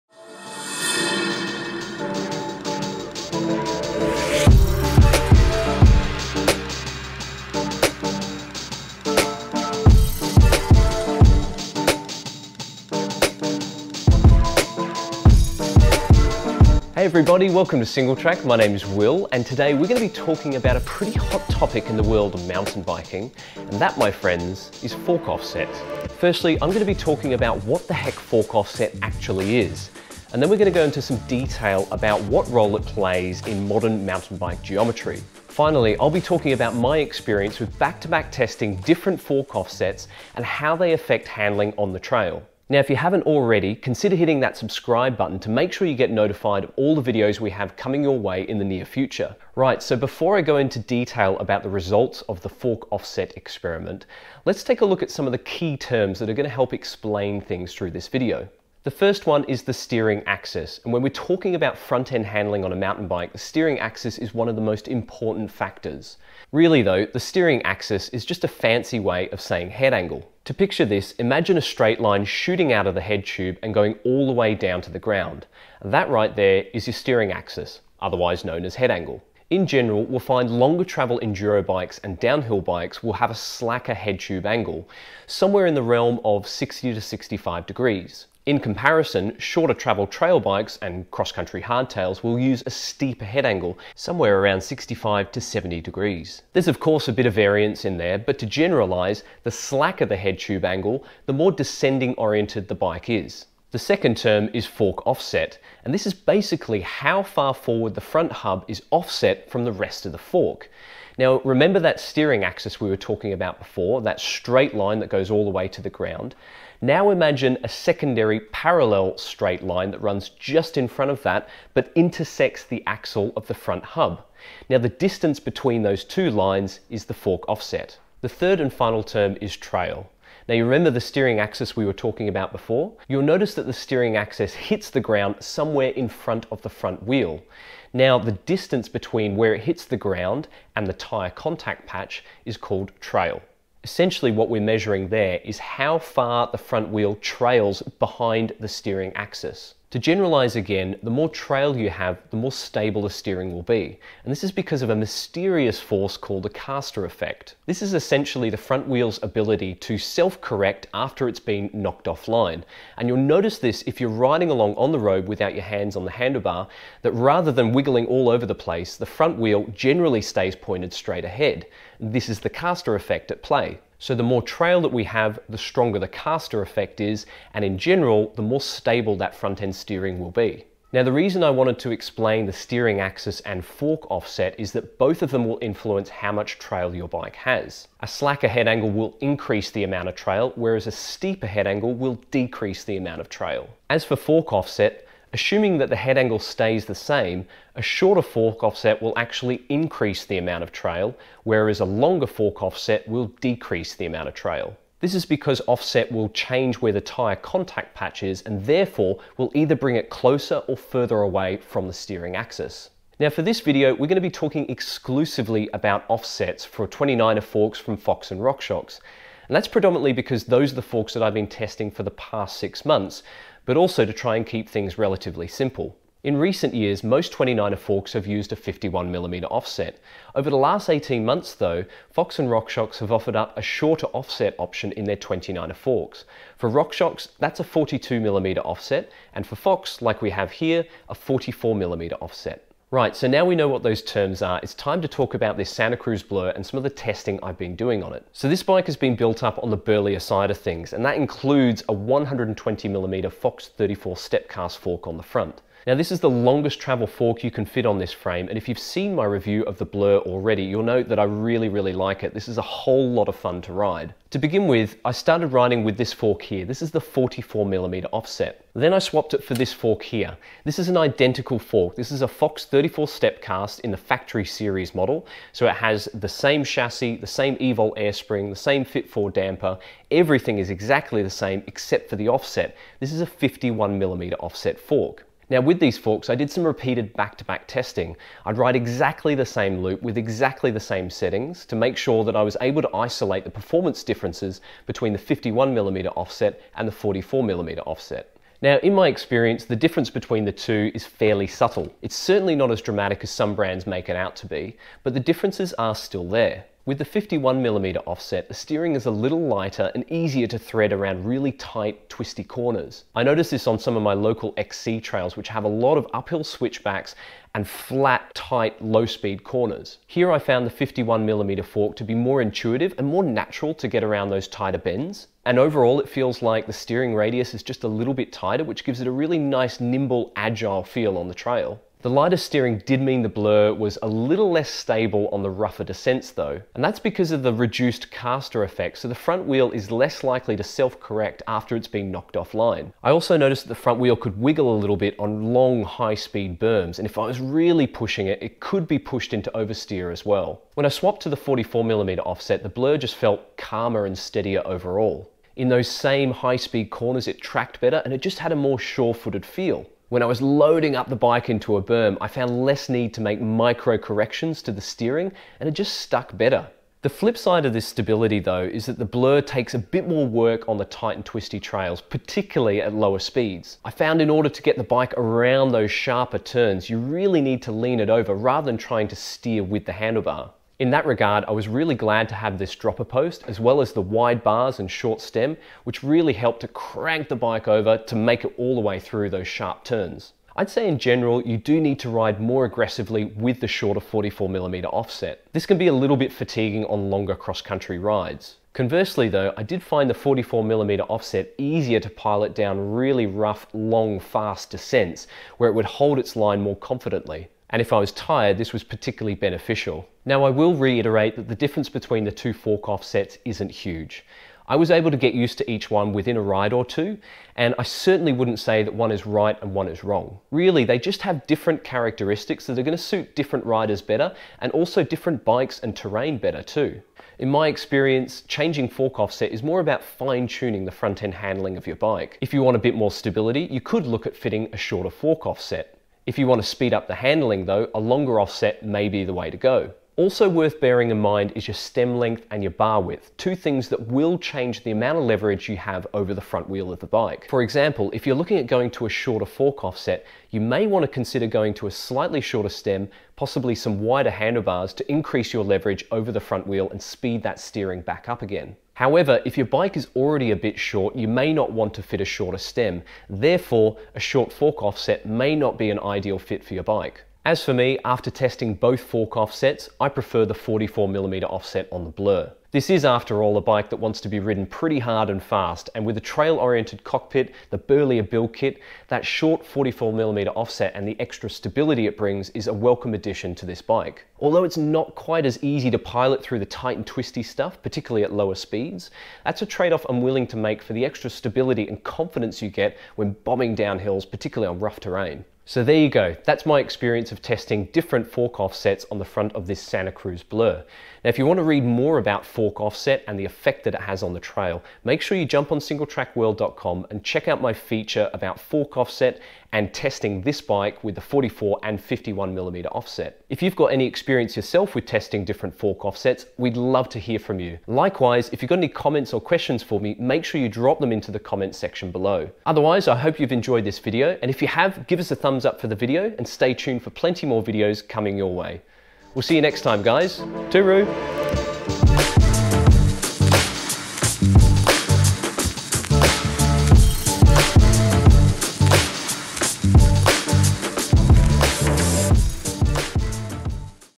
What is fork offset? | Testing The Santa Cruz Blur With 44 & 51mm Fork Offsets
Fork Offset - What is it? And why does it matter?

Unless you've been living under a firm piece of mineral sediment for the past two years, you'll have noticed that fork offset has been a particularly hot topic in the world of mountain biking.

Of course for many frame designers, fork offset – and the role it plays in governing a bike's handling – has been a consideration for a lot longer than that. But ever since Whyte launched its S-150 in early 2017, and Transition announced its range-wide 'Speed Balanced Geometry' around the same time, the collective swarm of journalists, internet forum dwellers, racers, marketing managers and consumers, have been buzzing about it a whole lot more.

The main topic in this conversation has been around the handling advantages (and disadvantages) of using 'reduced-offset forks'.

In this video, Wil goes into detail about what fork offset is and how it affects handling on the trail. Finally, Wil talks about his experience of back-to-back testing different fork offsets on the Santa Cruz Blur to determine which is best.

Got any questions about fork offset? Drop them into the comments section below and we'll do our best to answer them for you!